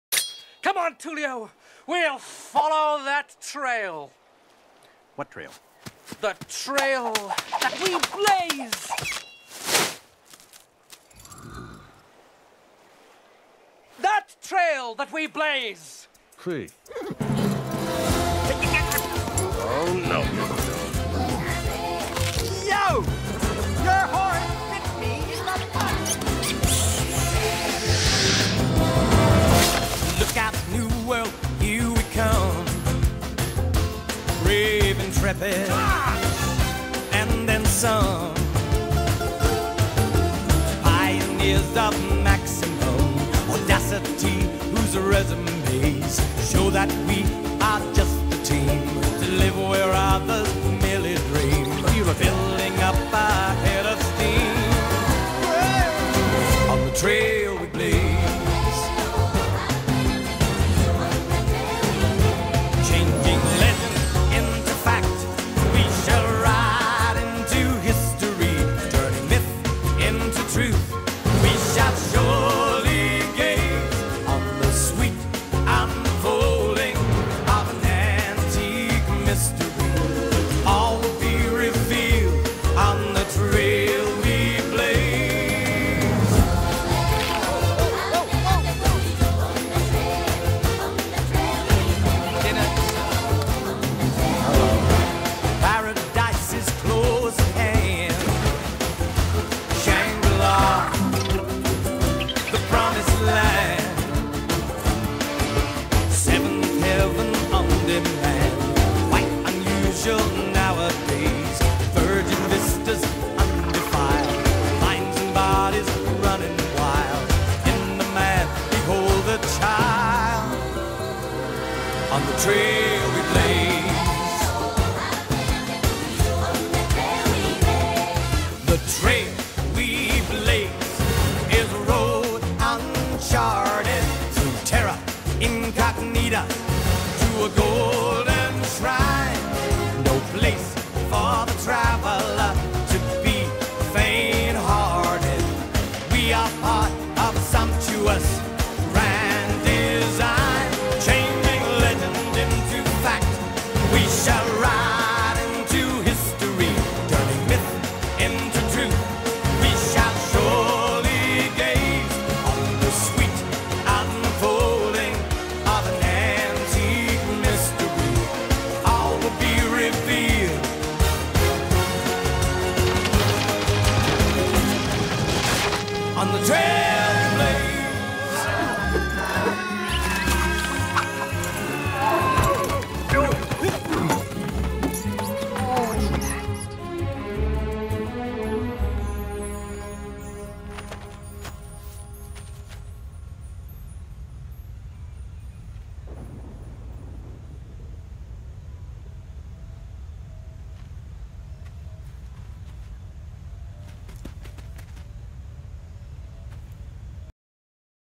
Tulio and Miguel goes DuoQ
Peeps Tulio and Miguel have found a map to El Gold'rado! Follow their steps, you might learn something with them!

Thanks to Arym and Miss Vodkita (Versag0) for the help when I needed it.